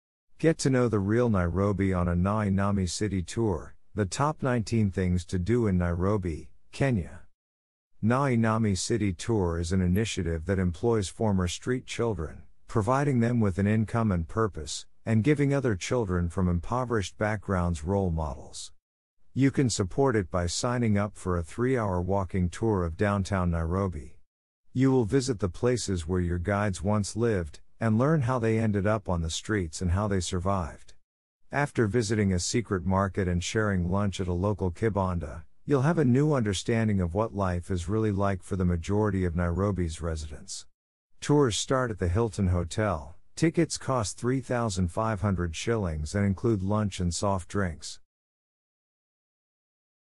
Get to Know the Real Nairobi on a Nai Nami City Tour
Get to Know the Real Nairobi on a Nai Nami City Tour - The Top 19 Things to Do in Nairobi, Kenya.
Nai Nami City Tour is an initiative that employs former street children, providing them with an income and purpose, and giving other children from impoverished backgrounds role models. You can support it by signing up for a three-hour walking tour of downtown Nairobi. You will visit the places where your guides once lived, and learn how they ended up on the streets and how they survived. After visiting a secret market and sharing lunch at a local kibanda, you'll have a new understanding of what life is really like for the majority of Nairobi’s residents. Tours start at the Hilton Hotel; tickets cost 3,500 shillings and include lunch and soft drinks. .